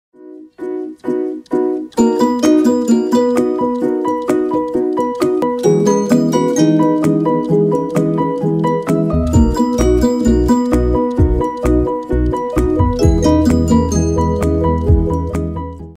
Red Fox Spring Strutting
Red fox walking along the roadside during a spring like winter afternoon.